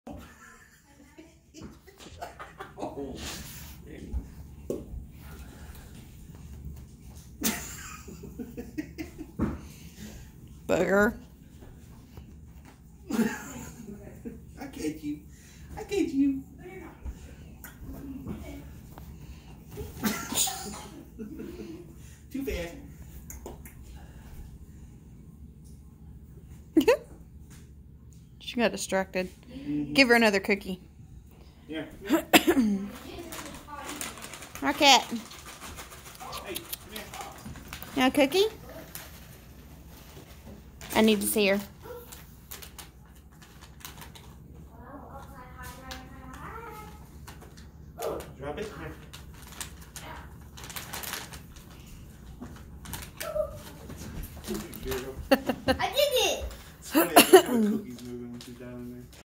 Roly Poly Raccoon || ViralHog
Occurred on July 22, 2020 / Johnson City, Tennessee, USA

"We rescued this raccoon baby at three-weeks-old when a dog brought it inside a friend's home. We bottle-fed her until she became I'll and passed away. She loved rolling and jumping and really loved her teddy Grahams. So when she wanted Teddy Grahams, she would 'play' like this with her 'daddy'."